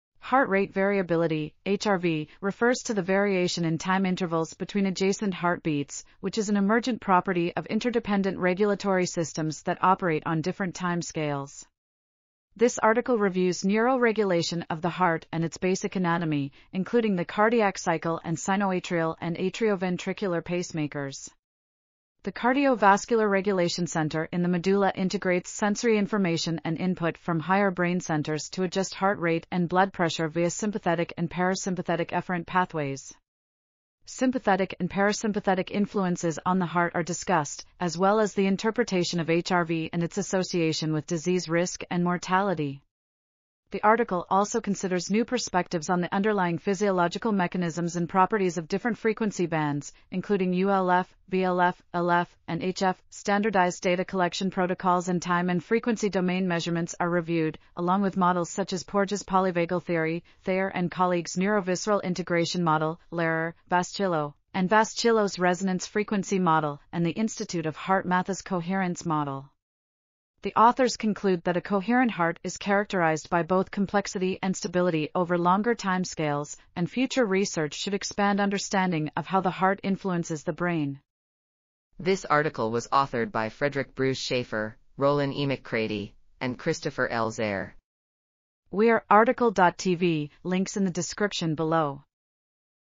A healthy heart is not a metronome: An integrative review of the heart’s anatomy and ... | RTCL.TV
### Keywords ###

### Article Attribution ###
Title: A healthy heart is not a metronome: An integrative review of the heart’s anatomy and heart rate variability
Authors: Fredric Bruce Shaffer, Rollin eMcCraty ,and Christopher L. Zerr

### Video Timestamps ###
0:00:00 - Summary
0:01:21 - Title
0:01:28 - Tail
0:01:32 - End